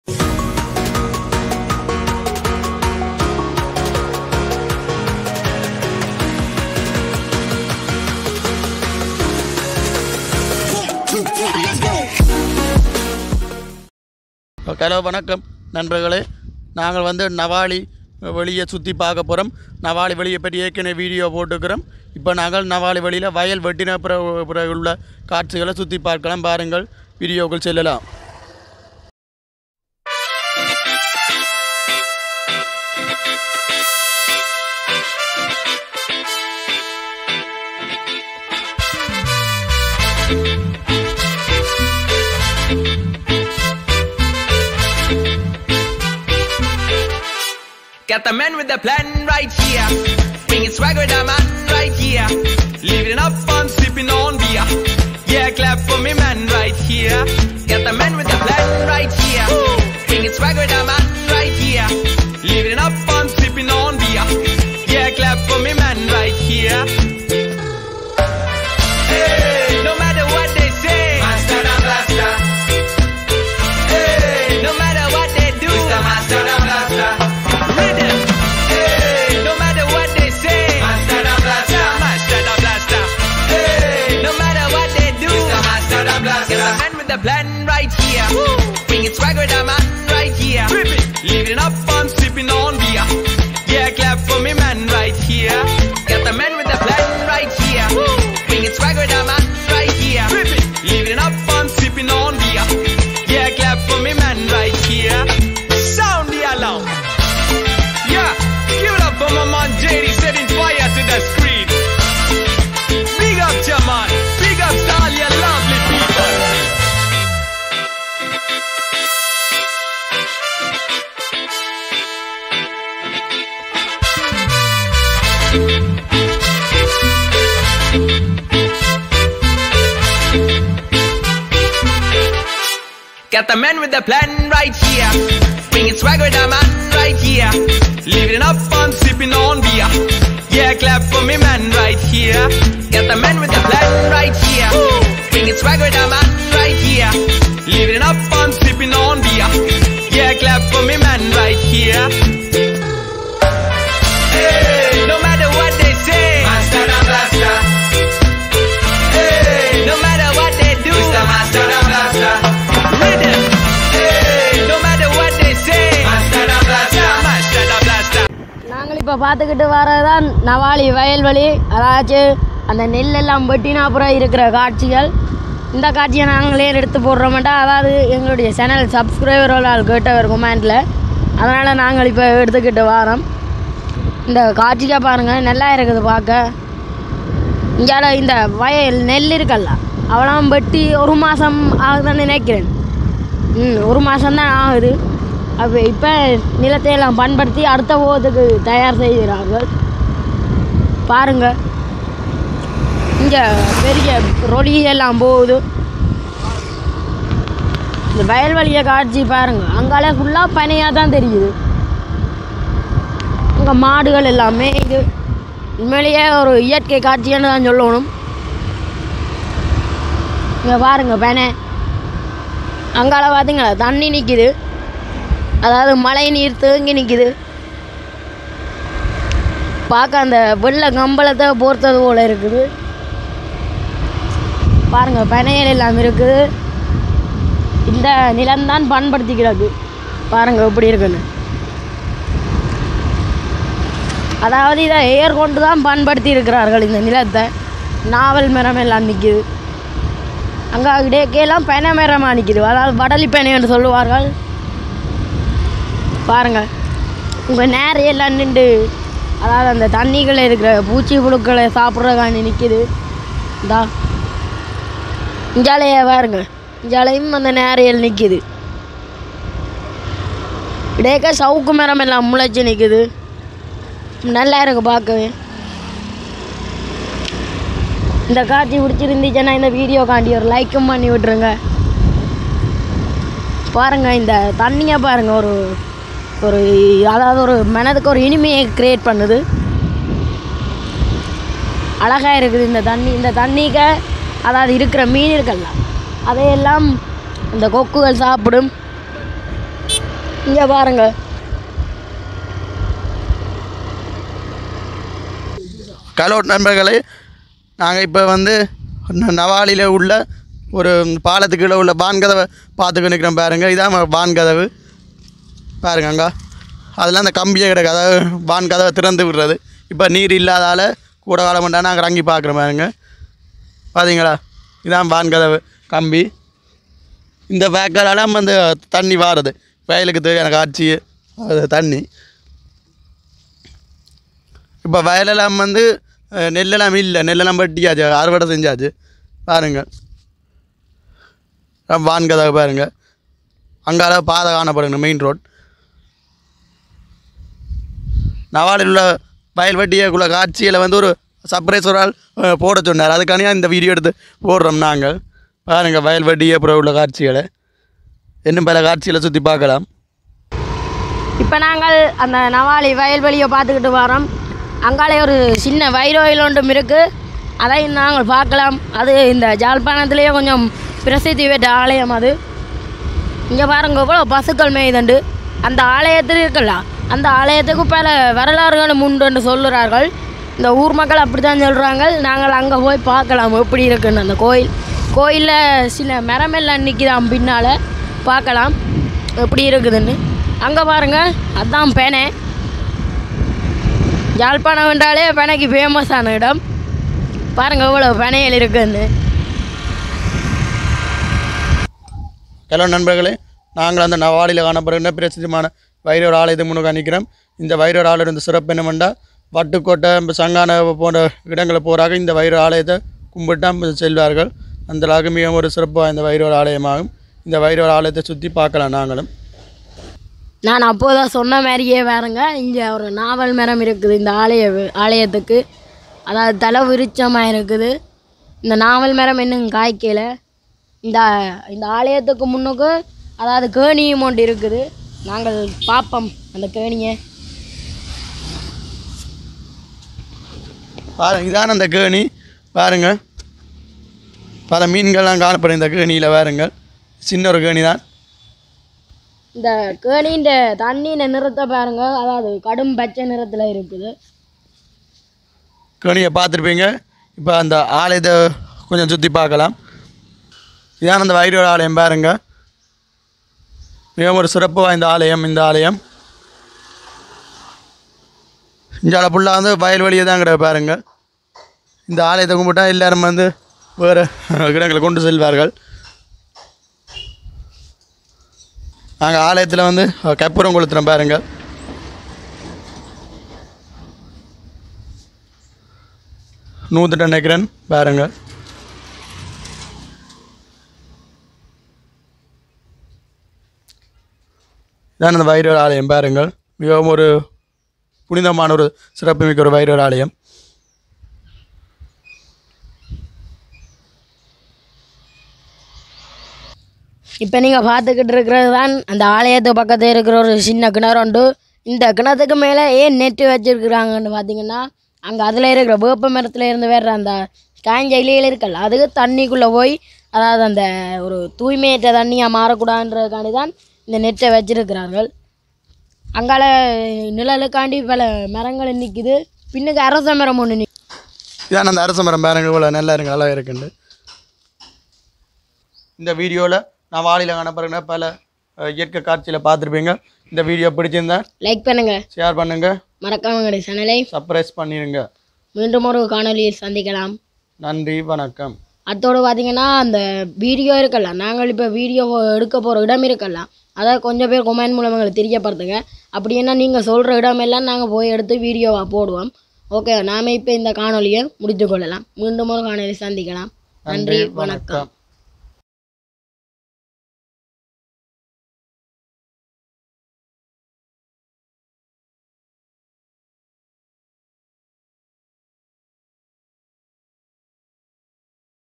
Village | நவாலி வயல்வெளி | Jaffna | Village life | Sri Lanka | Danistan | Kanistan | Beautiful
யாழ்ப்பாணத்தில் நவாலி என்ற இடத்திலுள்ள வயல்வெளி காட்சிகளை இந்த காணொலியாக பதிவுசெய்துள்ளோம்.

   💥  வாழ்க தமிழ் வளர்க உலகம் எல்லாம்💥